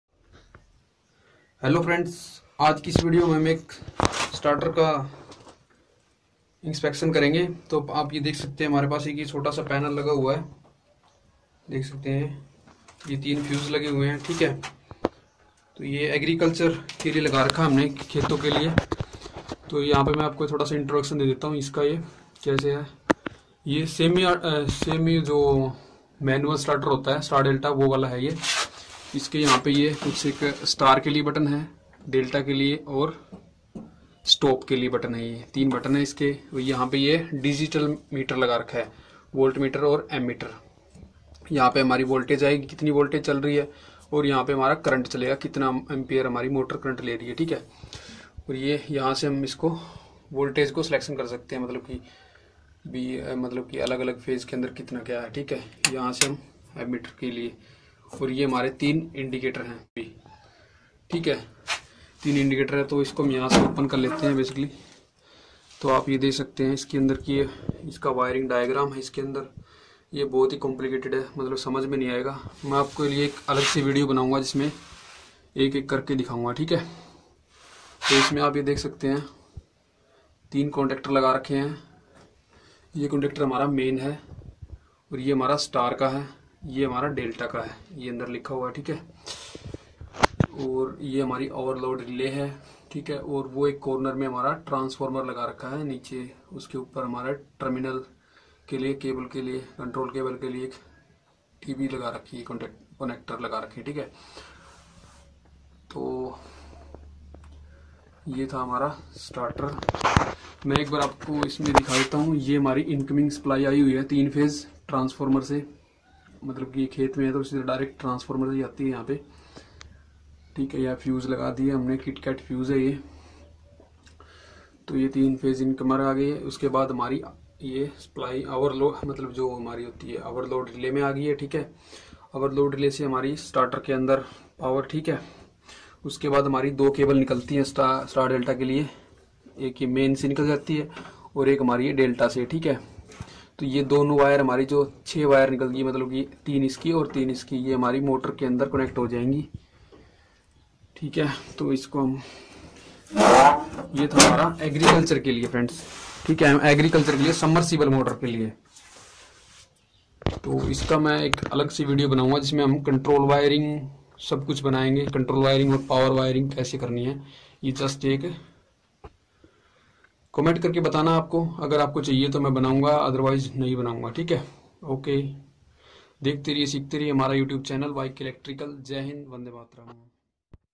Agriculture Submersible Motor Starter Rivew | YK Electrical
Hello friends today you watch here Agriculture Submersible Motor Starter , How to installed in field .